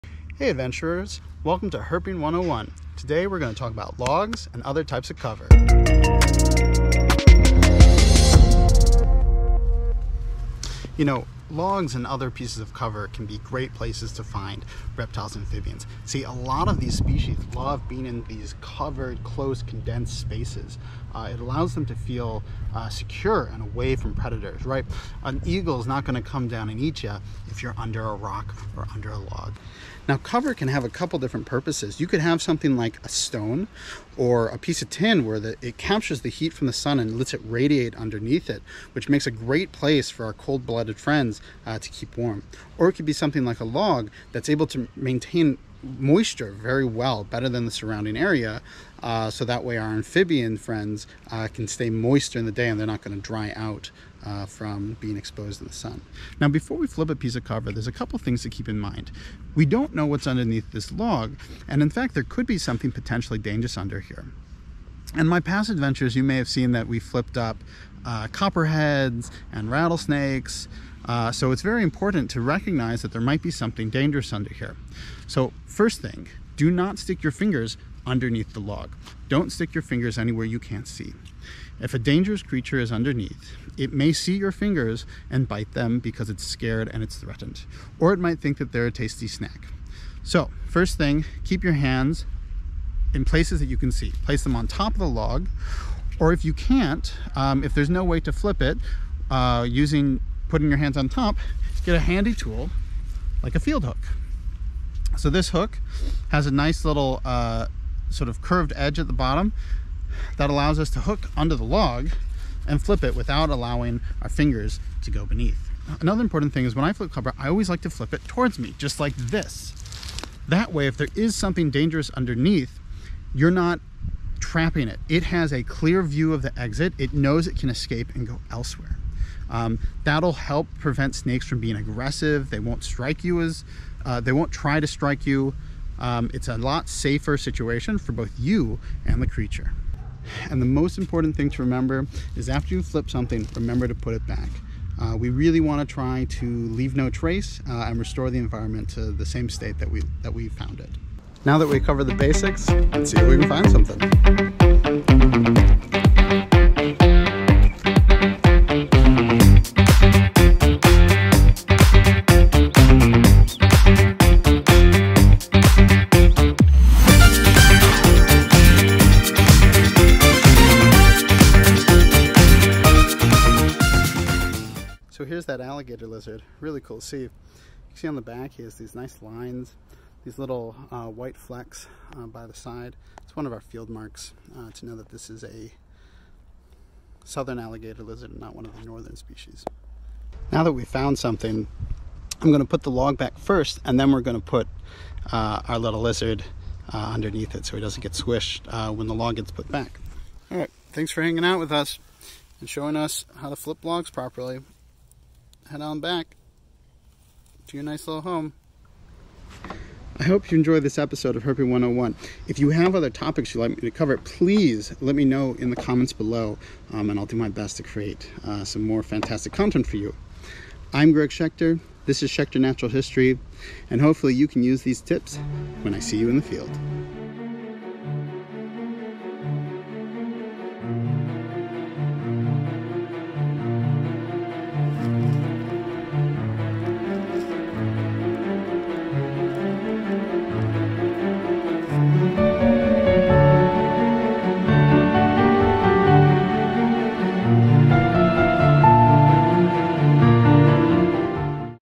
Herping 101: Flipping Cover
Lots of reptiles and amphibians love being in tight closed spaces.  Flipping logs, rocks, and other things can be a great to find these amazing creatures.  Watch to learn a few tips on important things to keep in mind while flipping.